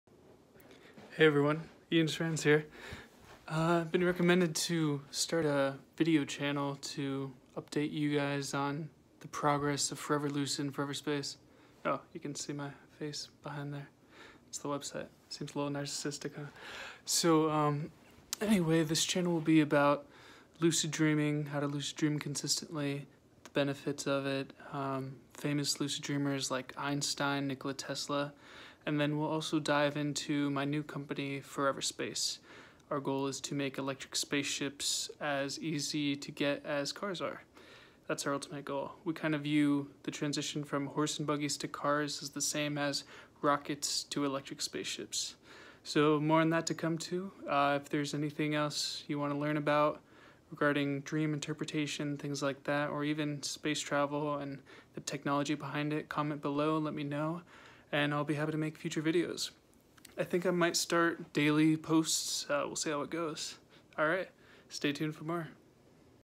ANOTHER ONE! | Ian Forever Lucid
Hey Everyone just another intro video. Forgot I made one last year. Looks like my goal is to start daily posts.

ForeverLucid.com